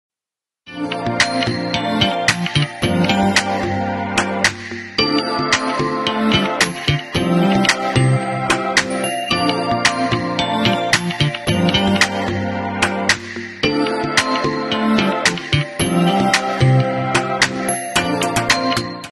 Beautiful butterfly buttons at Hansons
These hand-painted antique buttons are simply stunning... up for sale in our next costume auction